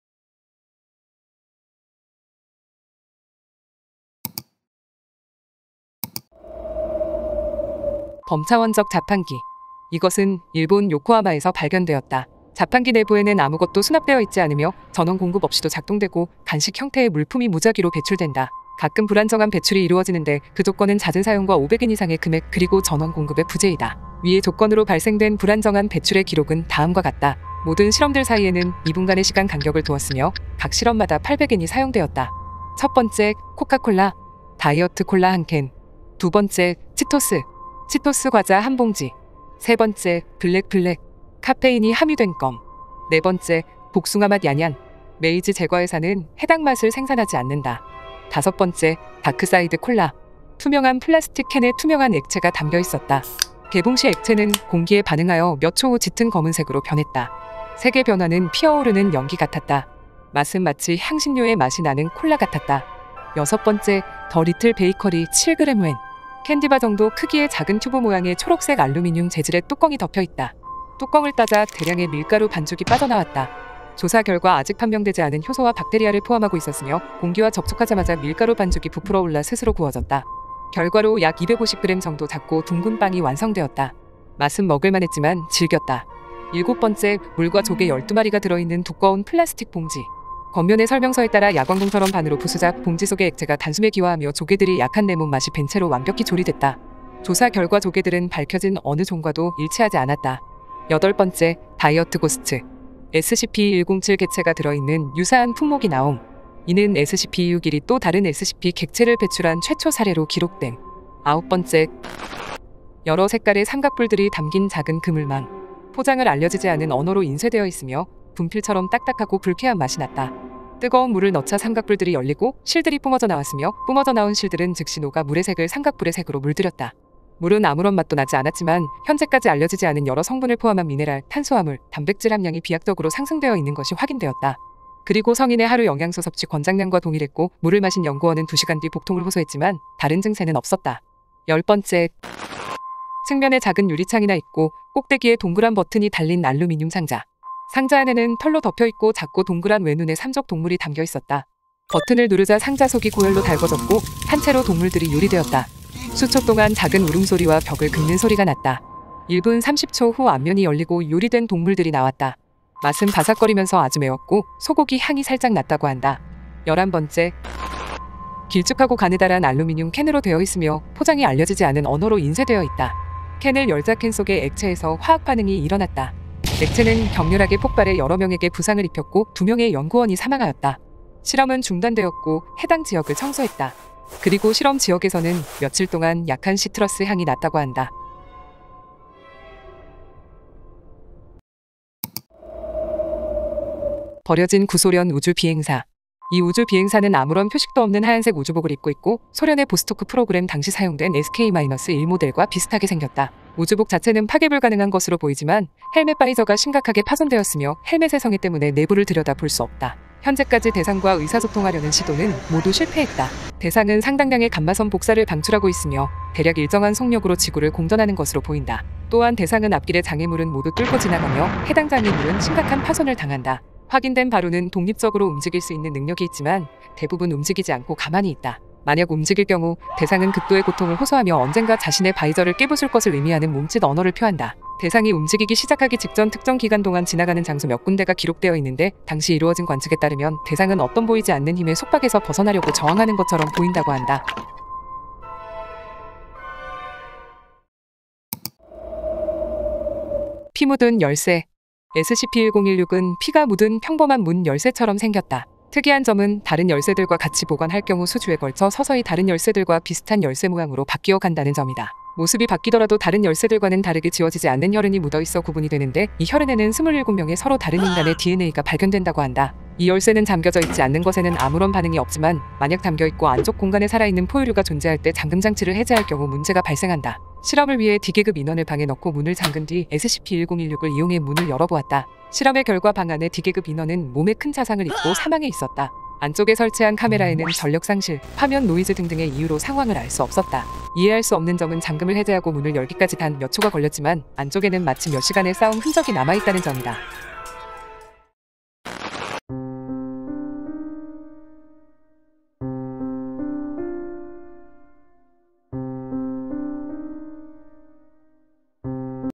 [SCP] 괴이한 자판기에서 나오는 괴이한 물건들. SCP- 261 (SCP-1959, SCP-1016)
0:06 SCP- 261 범차원적 자판기
3:12 SCP-1959 버려진 우주비행사
4:08 SCP-1016 피묻은 열쇠